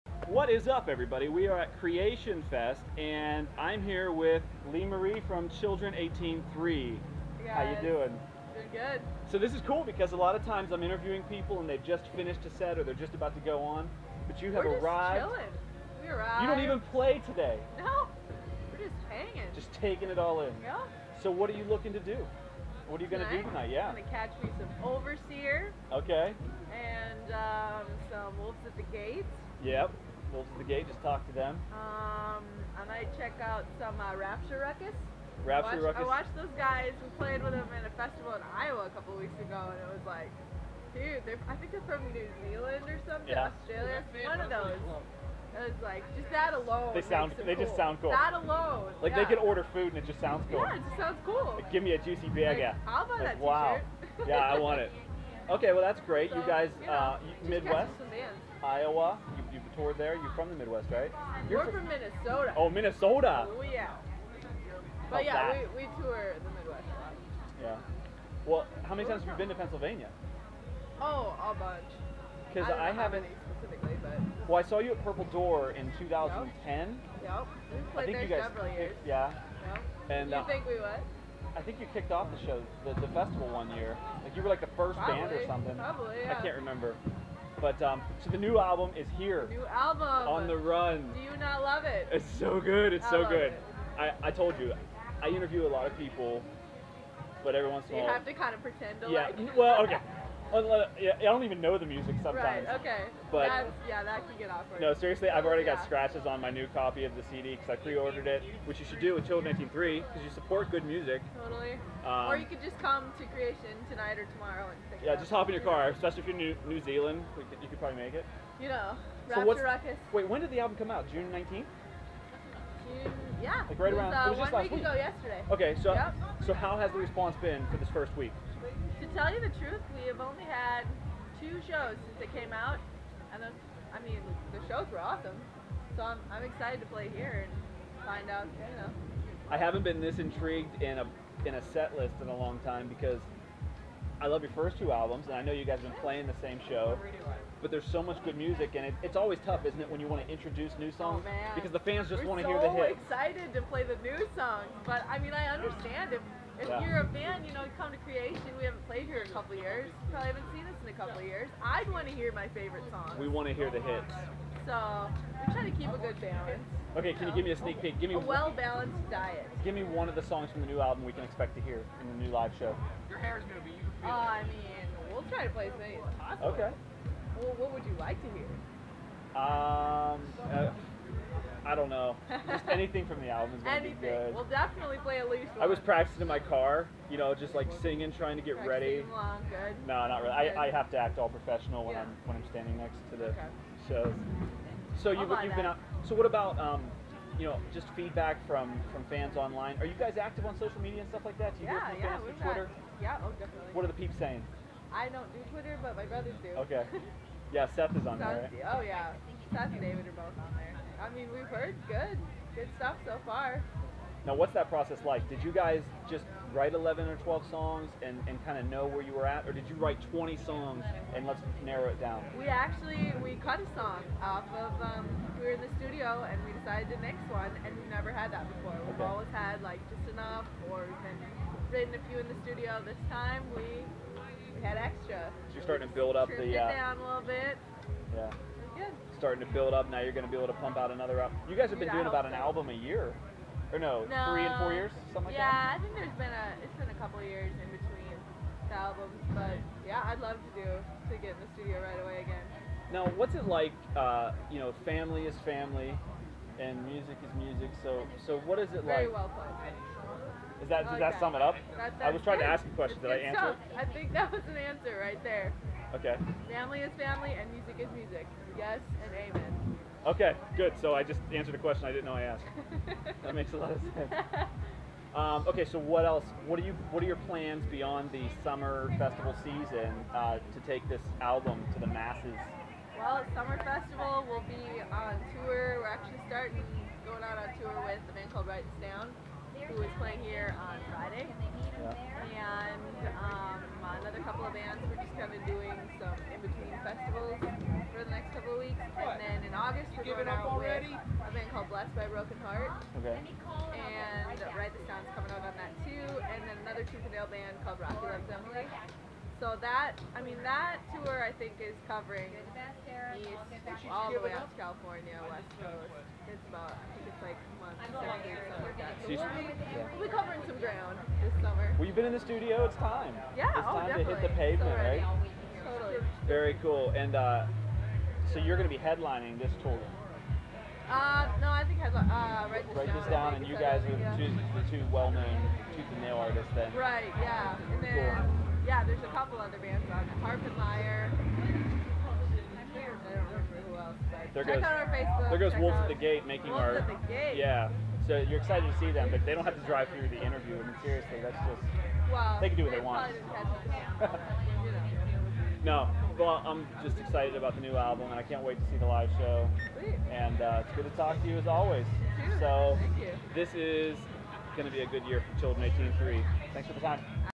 Christian Music Zine Interview of Children 18:3's Lee Marie
Clay Morgan interviews Children 18:3's Lee Marie Hostetter for Christian Music ZIne